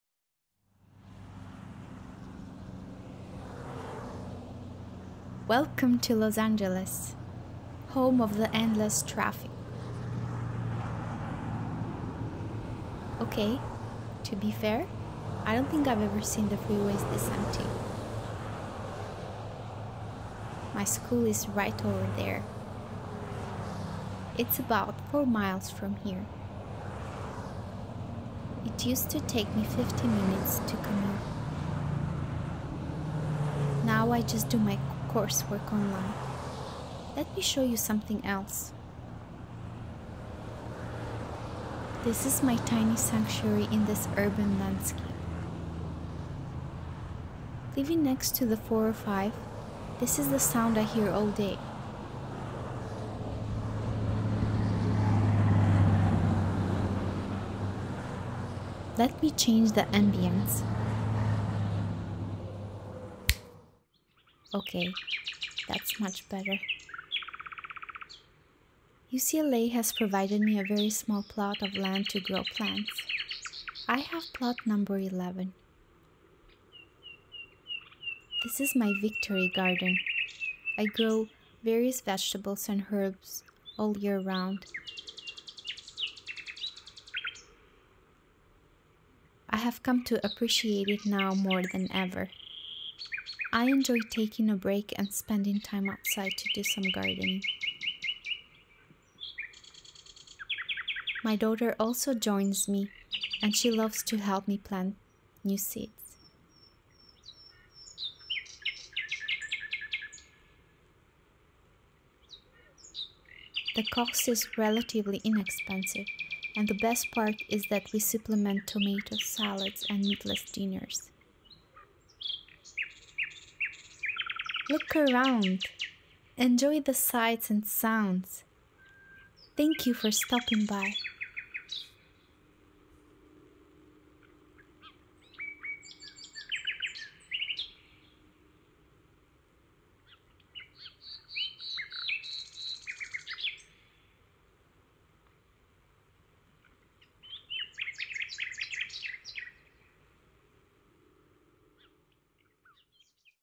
Victory Garden - 360 photos with audio- storytelling
360 photos with audio as a medium for storytelling.
For best experience use headphones and a VR headset such as an Oculus or any google cardboard headsets.

Busy motorway, traffic passing with occasional pause (M4, Slough) - Feb.1977 (1T11, reprocessed)